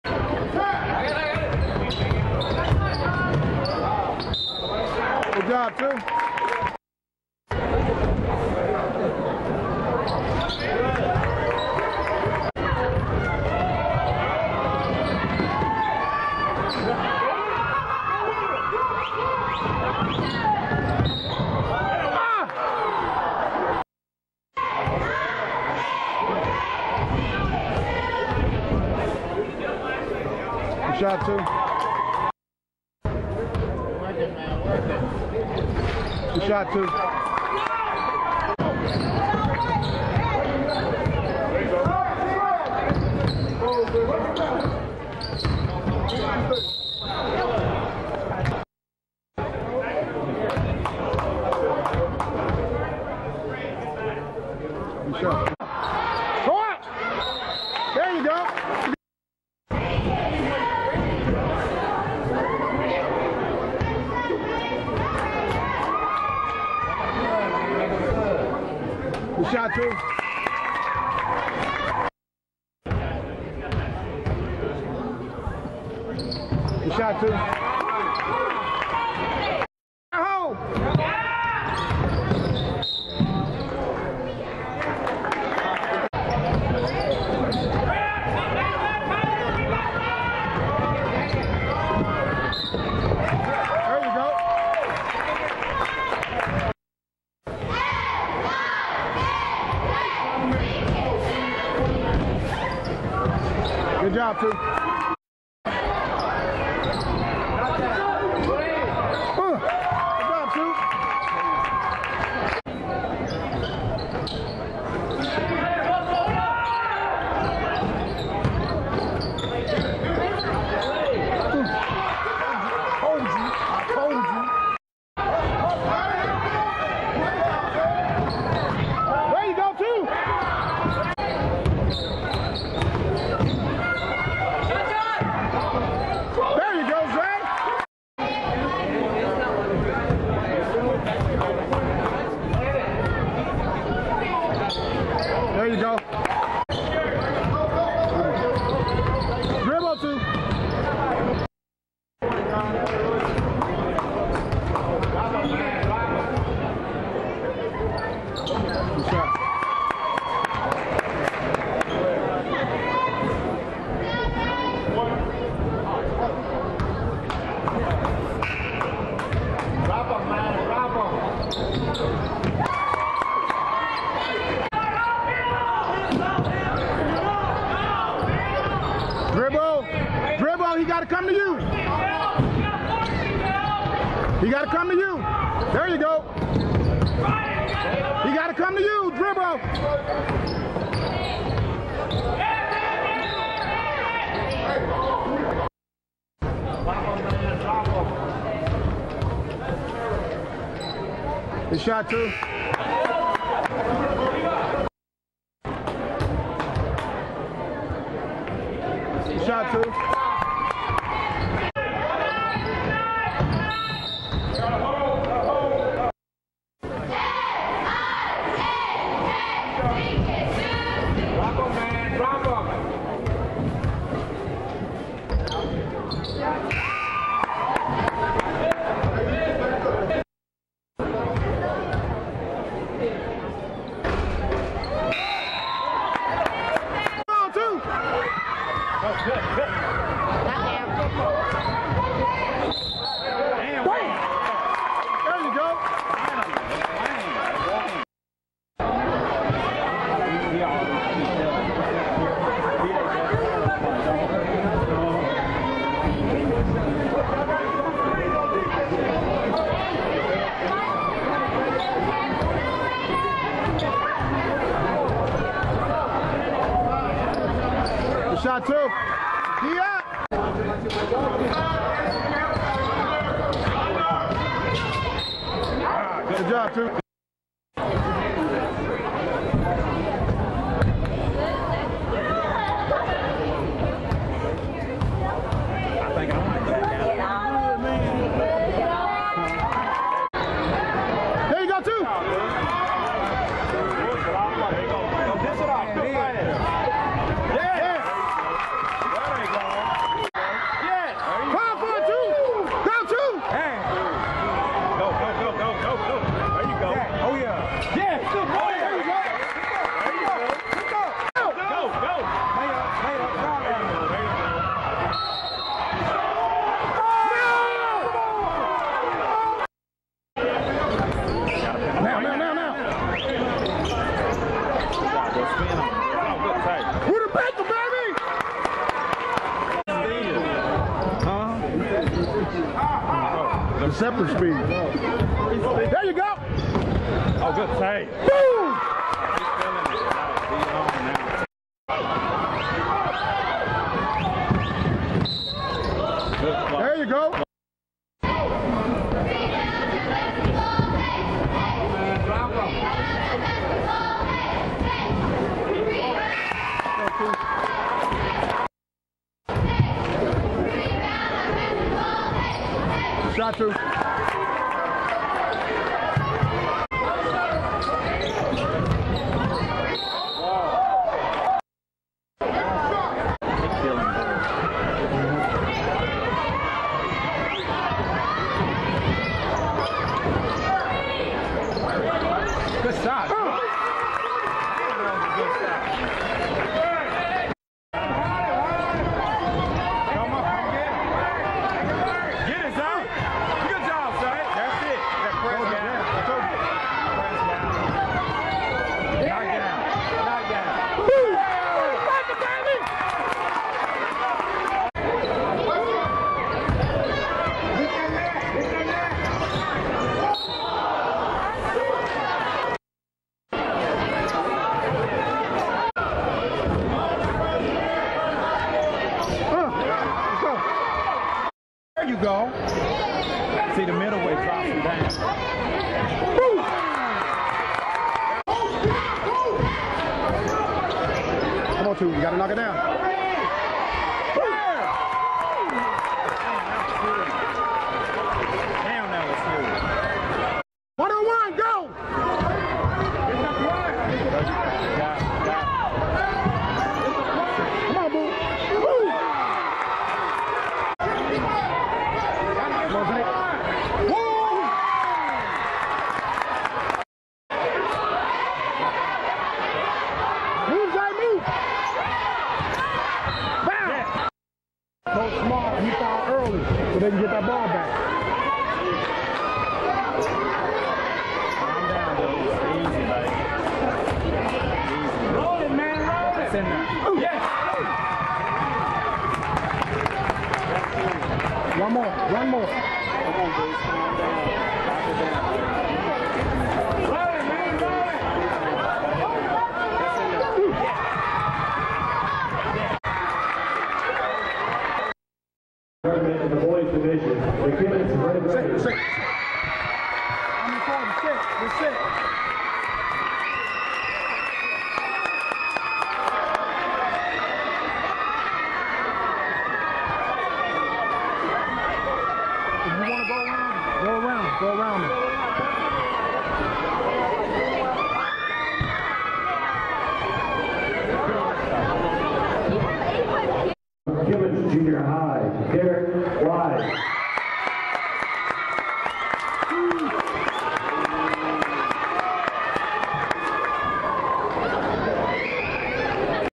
Derrick "Deuce" Wise 2015 Kenny Whitson Hi-Lites
Derrick "Deuce" Wise- The only 8th Grade All Tournament Player,  putting on an All-Star Performance at the 9th Grade 2015 Kenny Whitson tournament in Fort Smith, Arkansas @ Northside High School. (16 ppg avg.